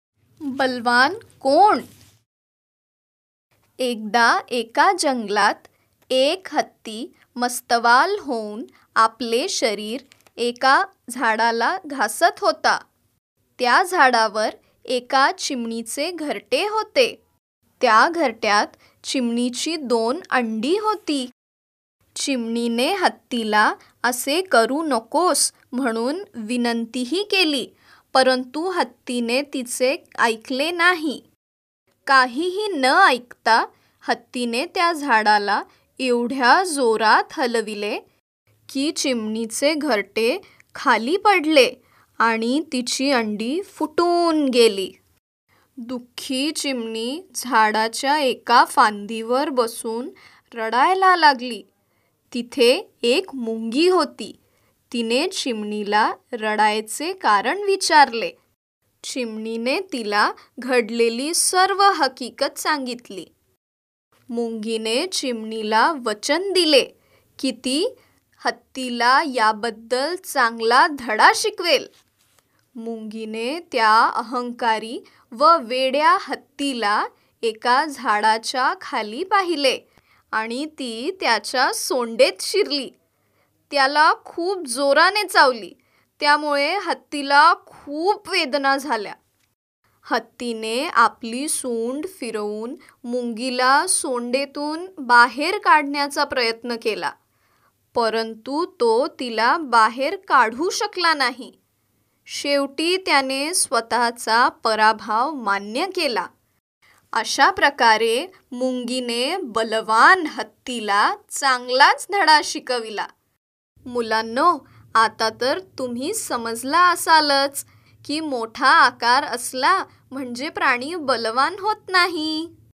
Balwan Kaun || Marathi Story
There was one strong elephant in jungle. one day elephant was having
itching so he started doing on tree so there was chimni there who have just lay there eggs on that tree because of elephant eggs got fell from tree.